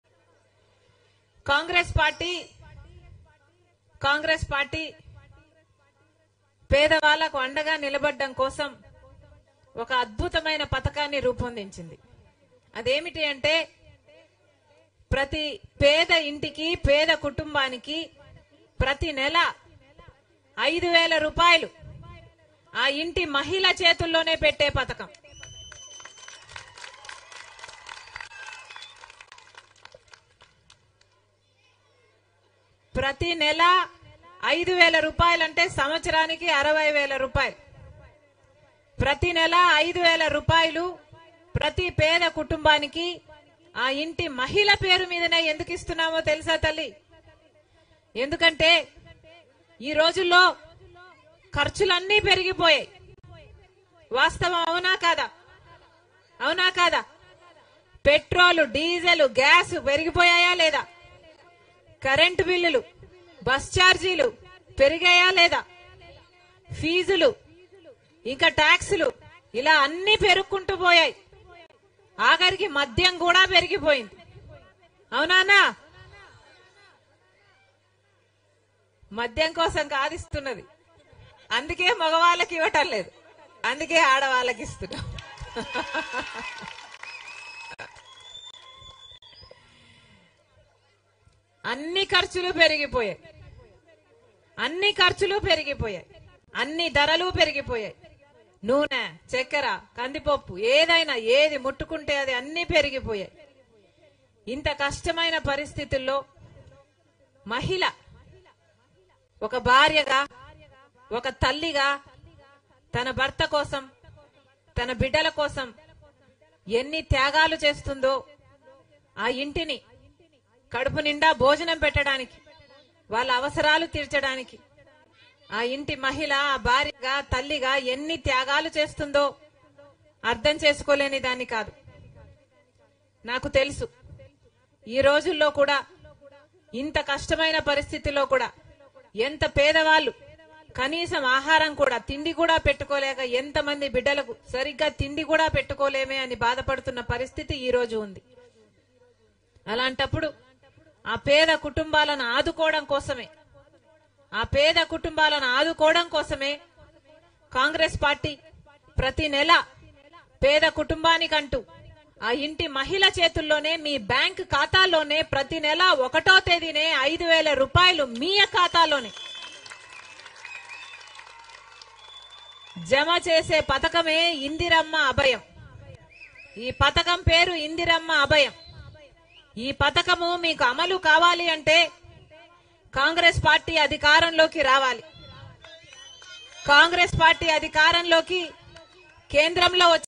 ప్రతి పేద కుటుంబానికి 5వేలు.. ! | YS Sharmila | AP Congress | Telugu TV
LIVE : వైఎస్ షర్మిల సంచలన ప్రెస్ మీట్ | APPCC YS Sharmila Press Meet | Telugu TV